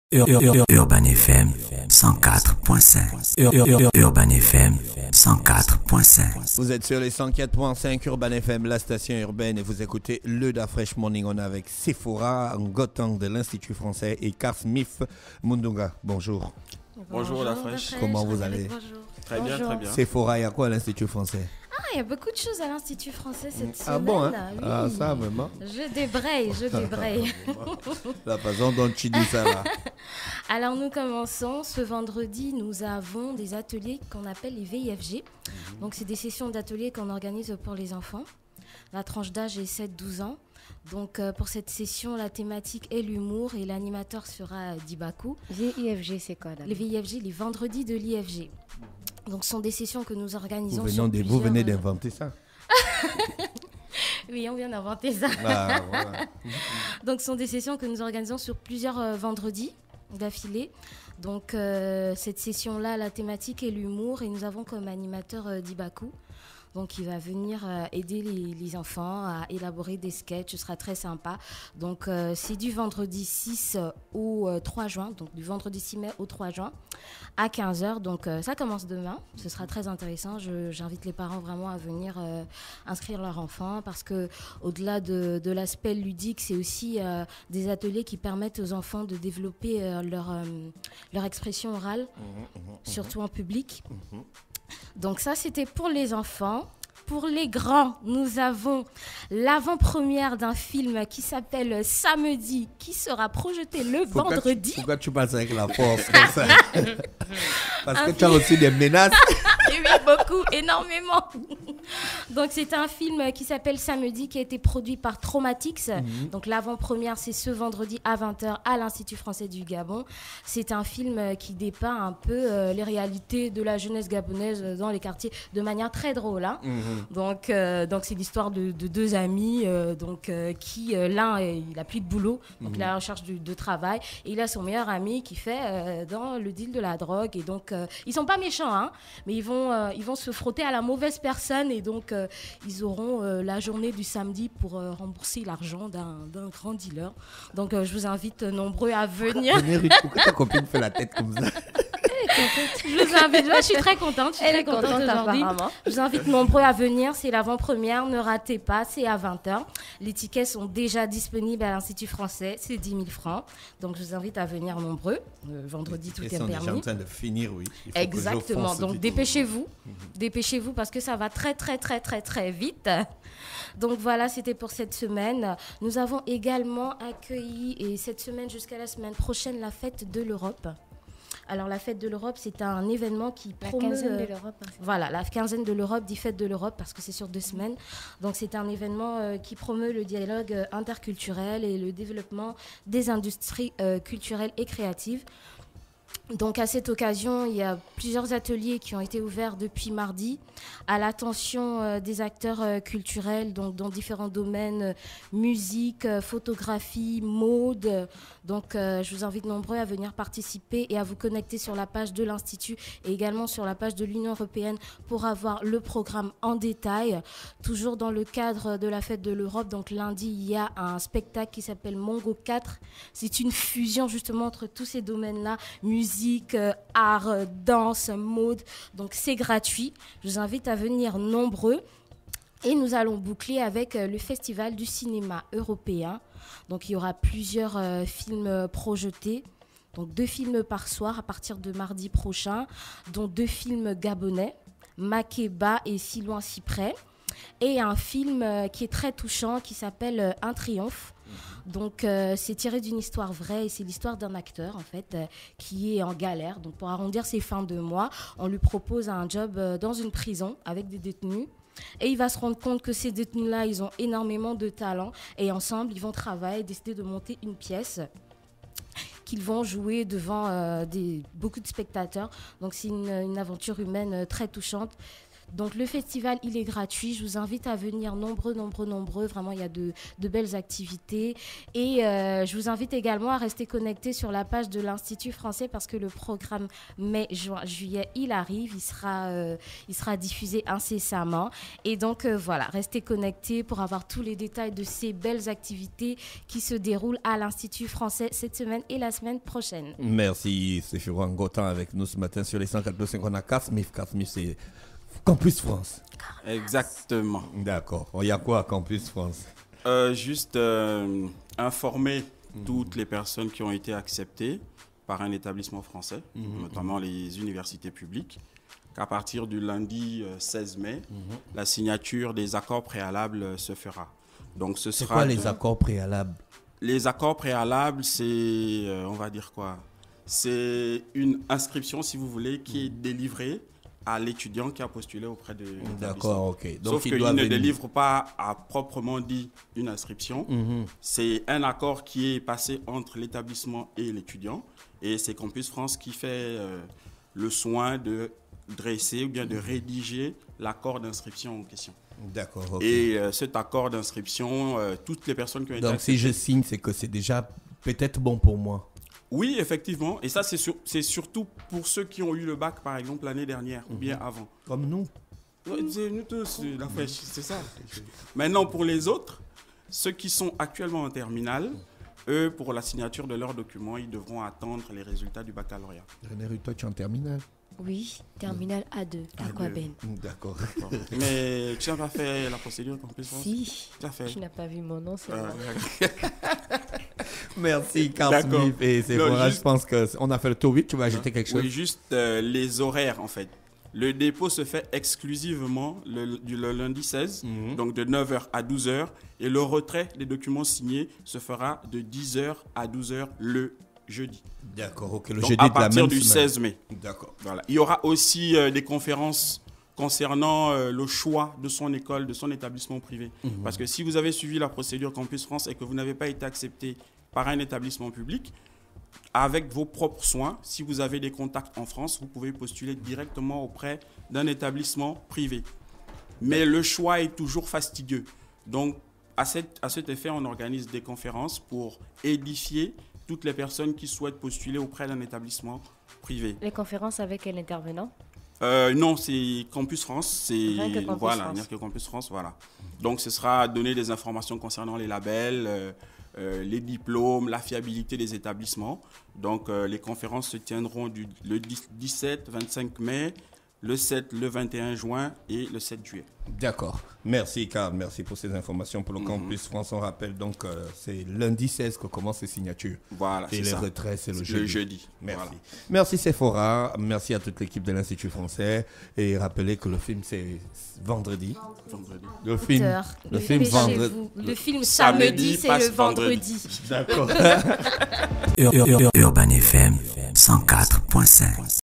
Sephora NGONTANG et Carl Smith MOUNDOUNGA parlent des activités culturelles de l'Institut Français
Suivez toutes nos actualités en temps réel et découvrez des images, des vidéos et des news exclusives!!!-